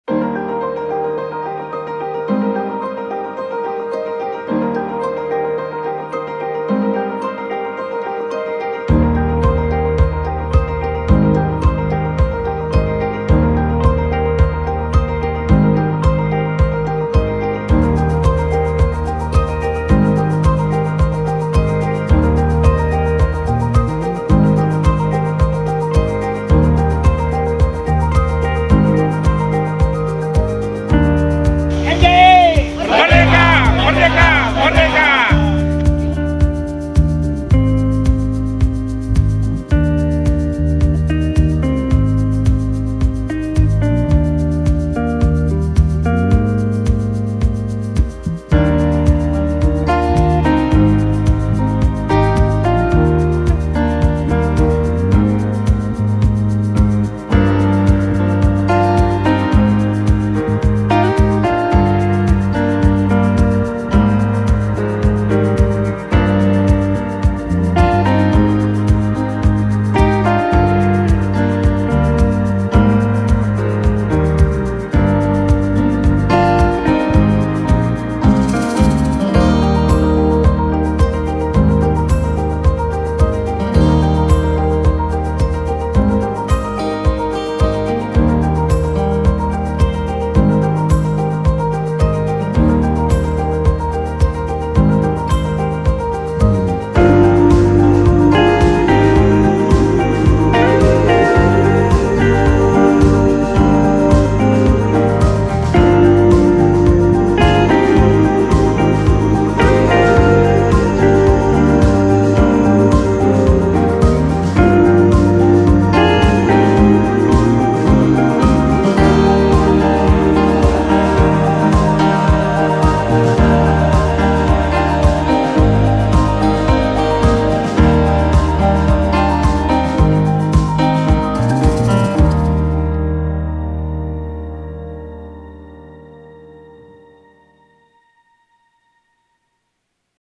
MJI MERDEKA - MERCEDES JIP INDONESIA
Mercedes Jip Indonesia,  family gathering MJI Merdeka ke Jatigede, Sumedang, Jawa Barat (6/8, 2022)